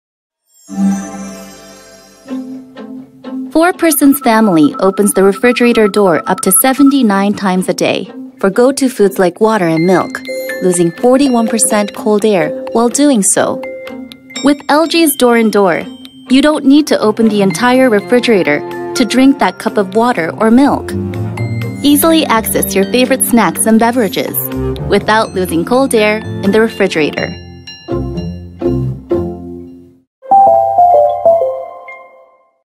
4091615_Bloque1
Gracias a las Neveras Door-in-Door™ de LG, no necesitas abrir toda la refrigeradora para encontrar los alimentos que consumes con frecuencia. Obtenlos fácilmente sin perder aire frío.